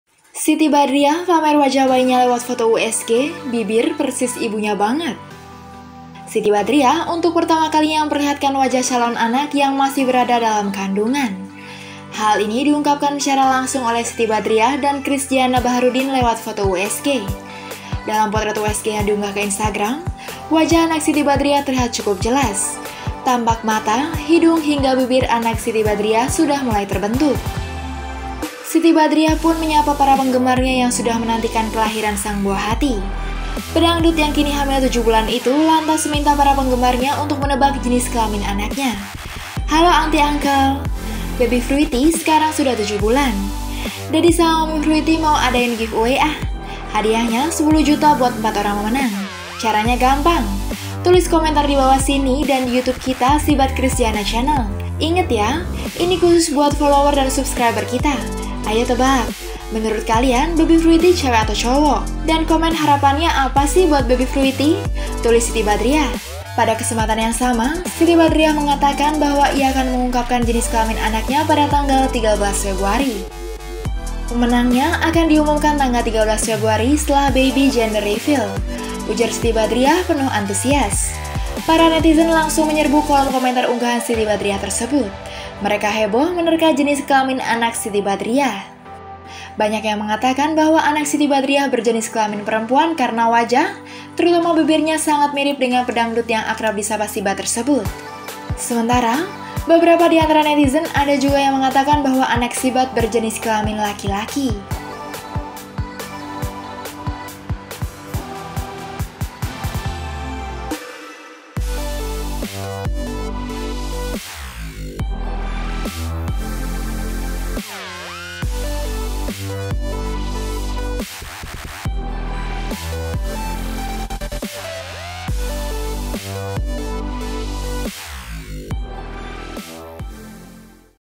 Siti Badriah Pamer Wajah Bayinya Lewat Foto USG, Bibir Persis Ibunya Banget
Siti Badriah untuk pertama kalinya memperlihatkan wajah sang calon anak yang masih berada di dalam kandungan. Hal ini diungkapkan secara langsung oleh Siti Badriah dan Krisjiana Baharudin lewat foto USG.

Dalam potret USG yang diunggah ke Instagram, wajah anak Siti Badriah terlihat cukup jelas. Tampak mata, hidung, hingga bibir anak Siti Badriah sudah mulai terbentuk.

Pengisi Suara / Video Editor: Anistya / Galih Fajar